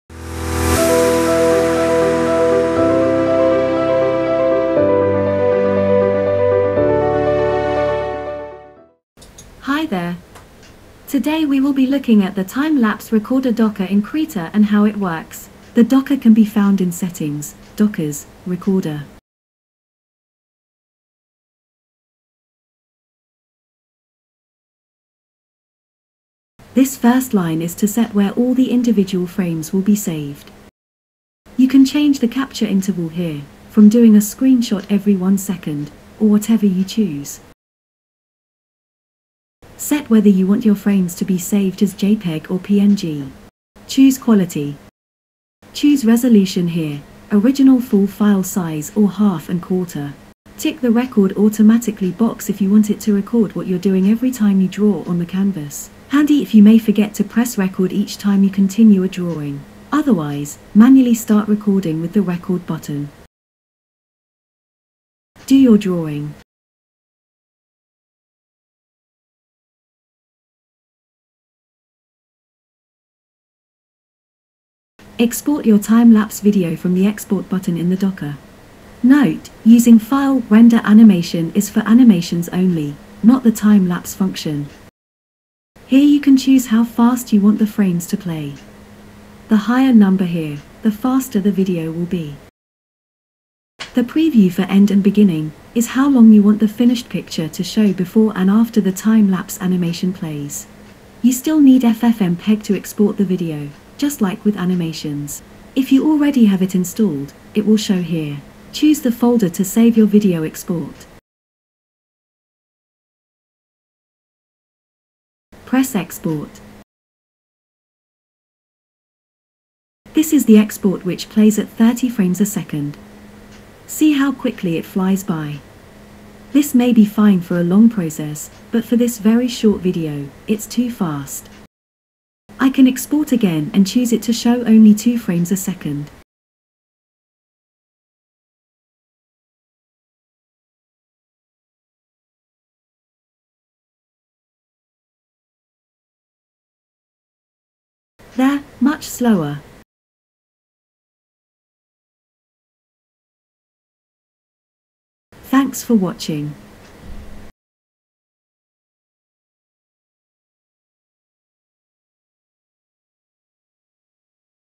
How To Use The Time Lapse Recorder in Krita
Simple overview of how to use the time lapse recording docker in Krita.